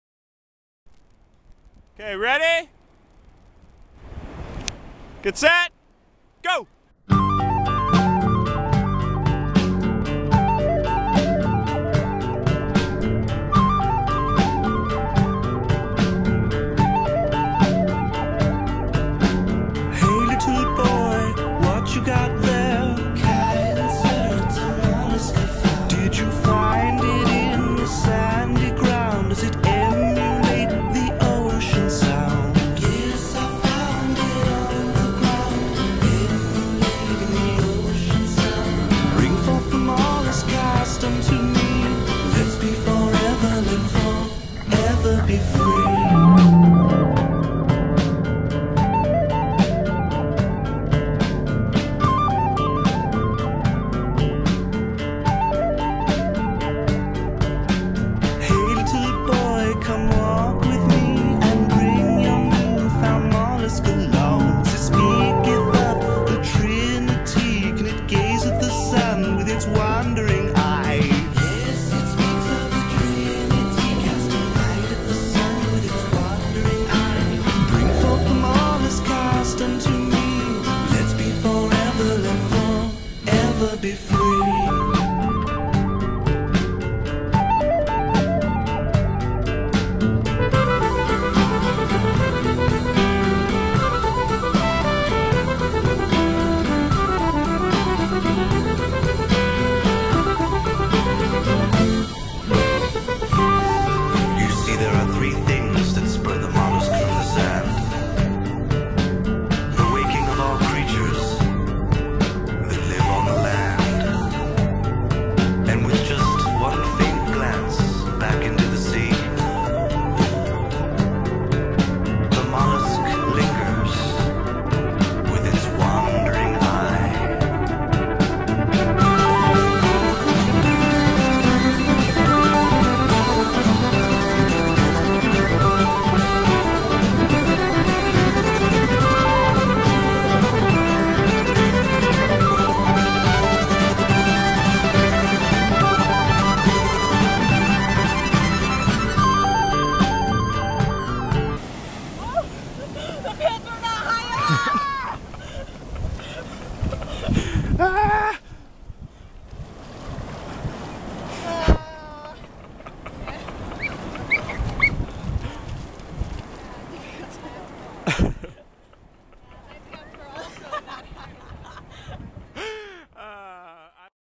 Beach Life in the Winter
A rare February day on the Oregon coast.